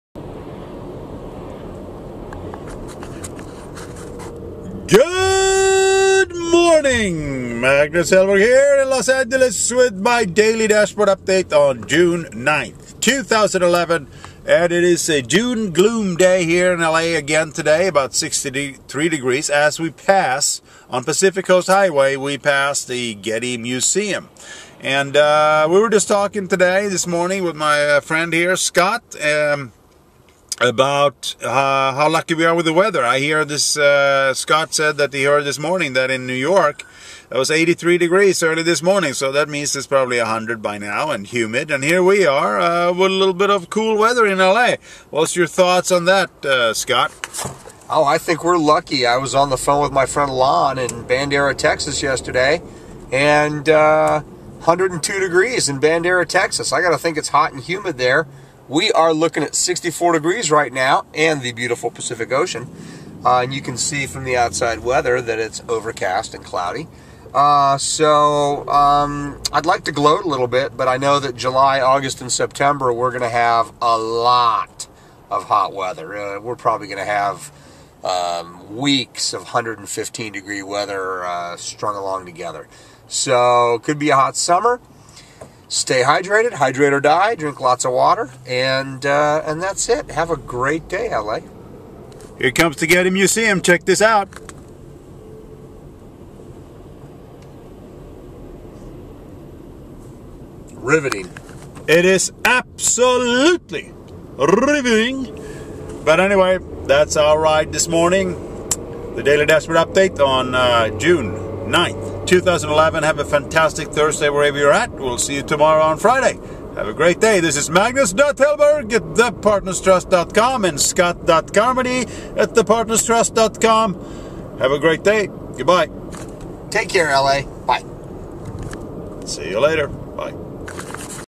Los Angeles Daily Dashboard Update, June 9th 2011, 61 F and Gloomy
Magnus Hellberg in Los Angeles Daily Dashboard Update. More San Francisco weather in LA. Passing the Getty Villa.